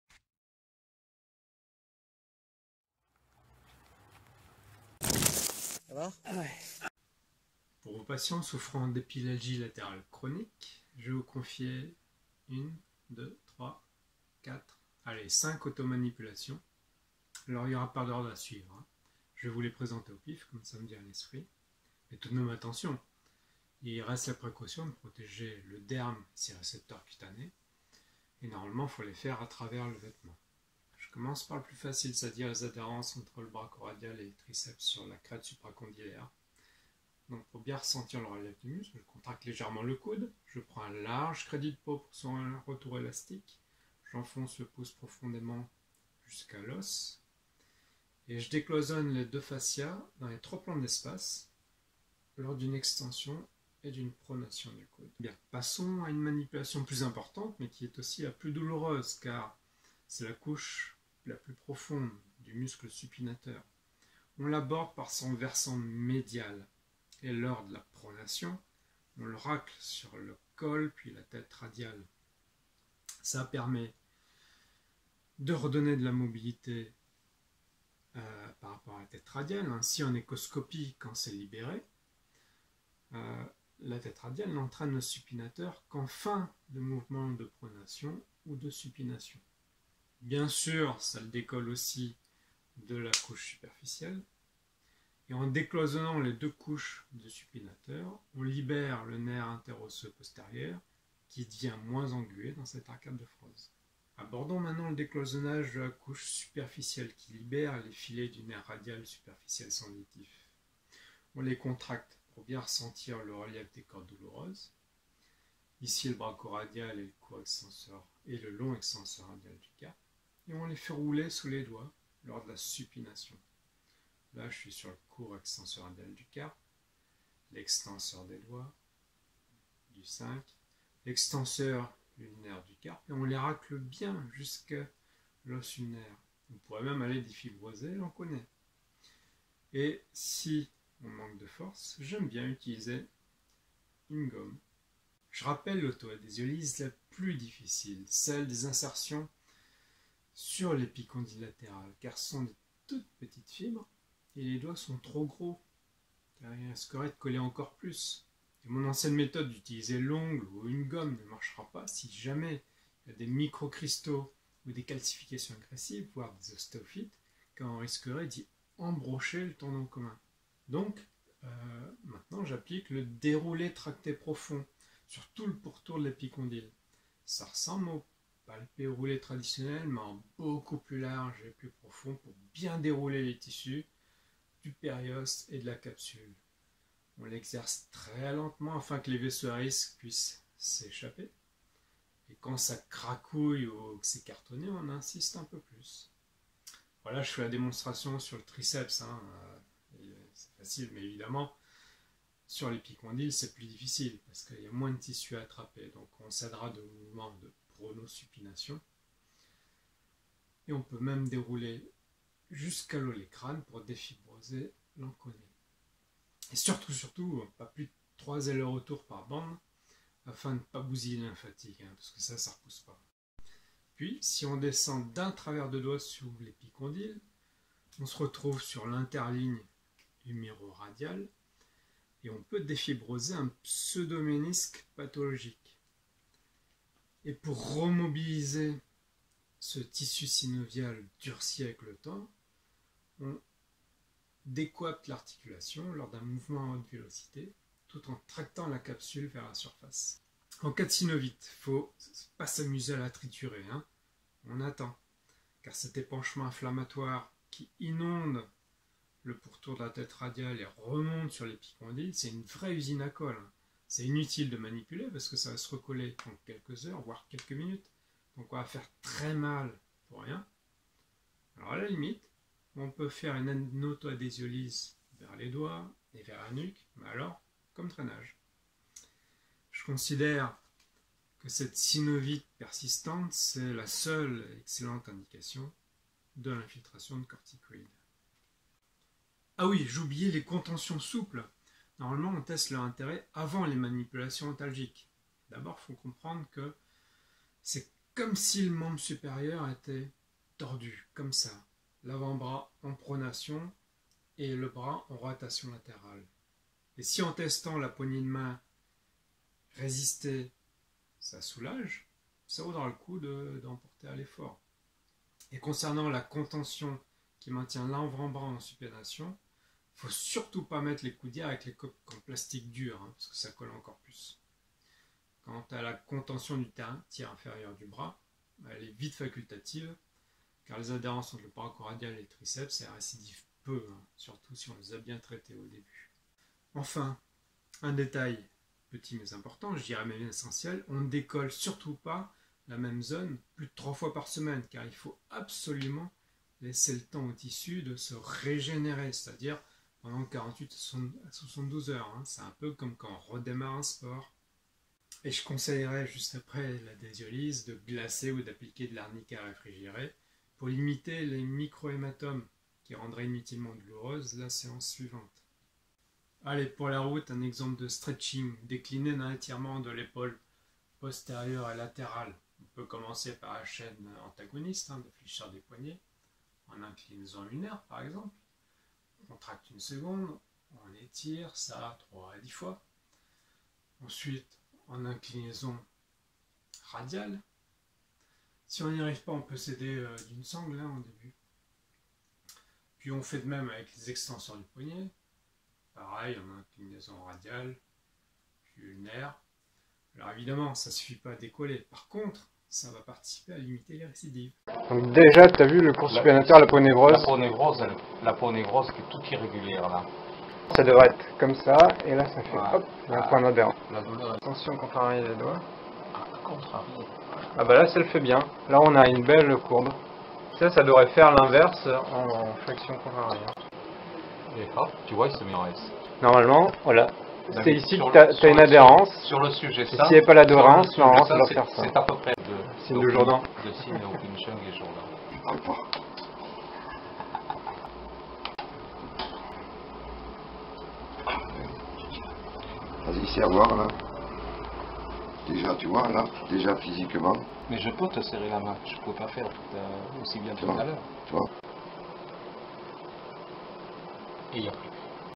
Épicondylite Latérale : Auto-Ostéopathie par Adhésiolyse Manuelle (tennis elbow)
épicondylalgie latérale chronique : 5 Auto-Manipulations
Prévention des rechutes, si MANIPULATIONS TESTS efficaces !
Contre-indications = SYNOVITE, peau fragile
1) "Décloisonnage" des Adhérences entre les Fascias Profonds des :
- Brachio-Radial / Triceps (sur la crête Supra-condylaire)
- Brachio-Radial / Long Extenseur Commun Carpe / épicondyliens
- Supinateur (autour de la Tête et du Col du Radius)
2) Pseudo-ménisque pathologique (interligne Radio-capitellaire) = Dépiégeage intra-articulaire
3) "Déroulage Profond" du Tendon Conjoint (sur l'épicondyle latéral)